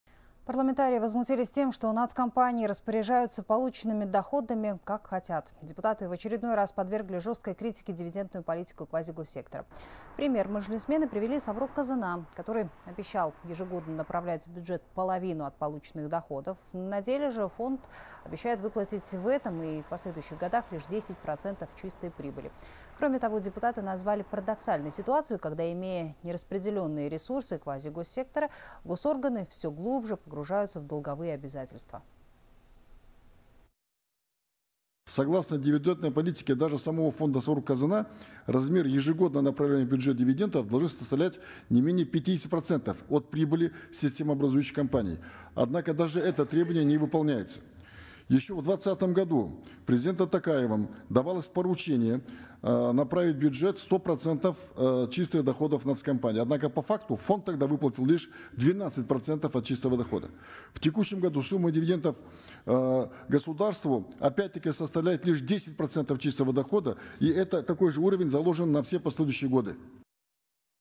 «На свое усмотрение» – депутат о дивидендах нацкомпаний
Парламентарии возмутились тем, что нацкомпании распоряжаются полученными доходами, как хотят. Депутаты в очередной раз подвергли жёсткой критике дивидендную политику квазигоссектора, сообщает корреспондент «Хабар 24».